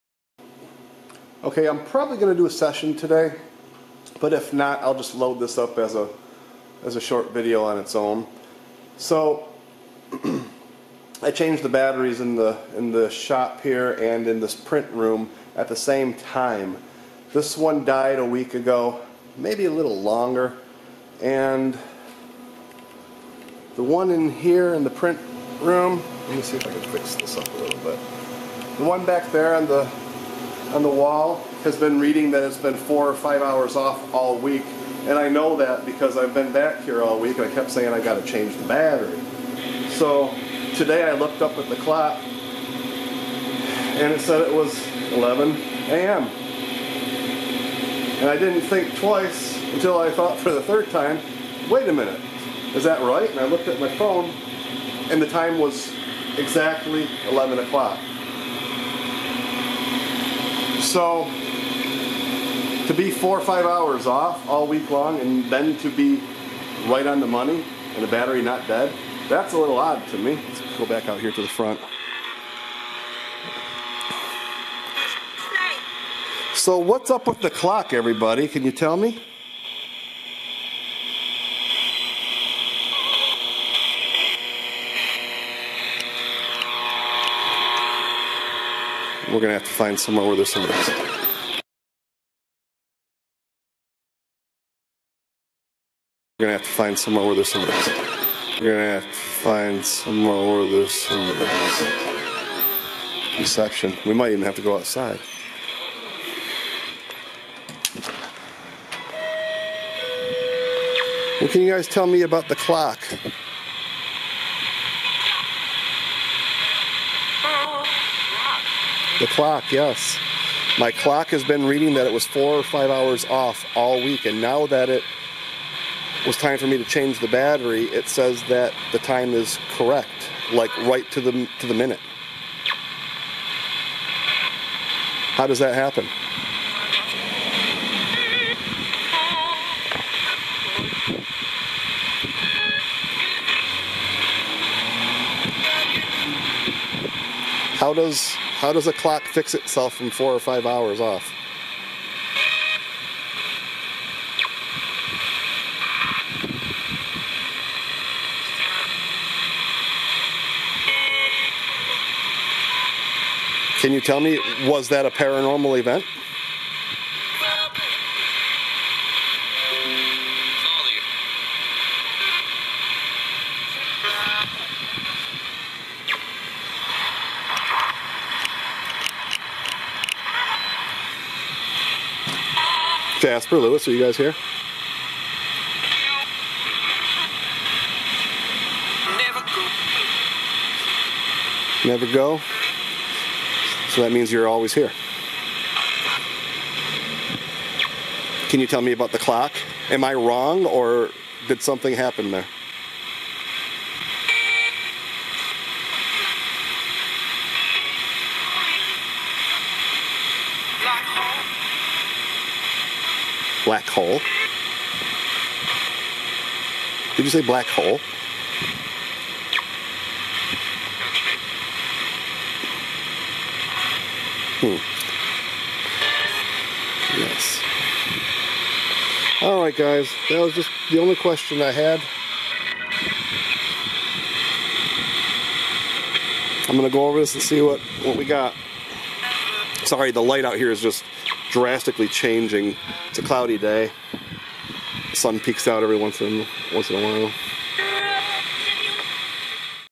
Spirit Box Session Paranormal Clock shenanigans?
Both clocks had the batteries placed in them the same day. The garage clock died and the clock in the print room fell 4 hours short. It was like that for a week before today when I decided I was going to change the batteries.

When I went in the room and looked up at the clock the time was exactly right!  Not even a minute off.  This is pretty strange . I do not understand how a clock could catch back up from being off 4 hours.